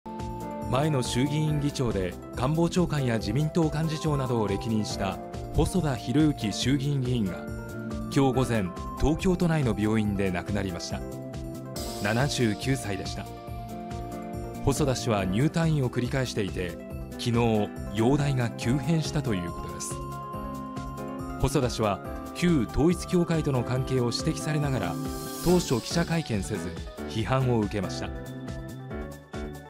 細田前衆院議長が死去官房長官や幹事長を歴任
前の衆議院議長で、官房長官や自民党幹事長などを歴任した細田博之衆議院議員がきょう（10日）午前、東京都内の病院で亡くなりました。79歳でした。細田氏は入退院を繰り返していて、きのう（9日）、容体が急変したということです。細田氏は、旧統一教会との関係を指摘されながら当初、記者会見せず、批判を受けました。